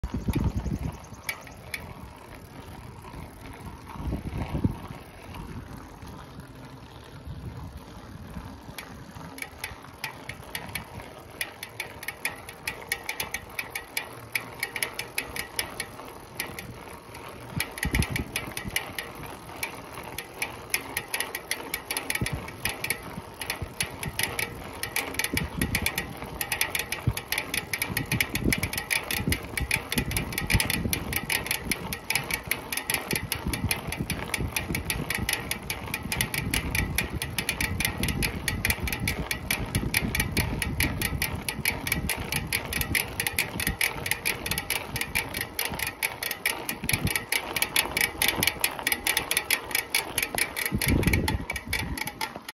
2021 Cannondale Scalpel HollowGram wheelset issue
This is brand new bike with new set of wheels. When rear wheel is under load ; going uphill most or all spokes make cracking sound where they cross each other .  They just advise to contact my local Cannondale bike shop. HollowGram wheel Customer MANUAL does not specify spokes tension or lengths or any parts for the wheelset only recommended tire pressure. and warnings. Wheels are new , never were abused or damaged in any way. Noise started on a 1st uphill and is terrible every time I want to go uphill. On straights or downhills wheels are quiet. I did not adjust any spokes on this wheelset.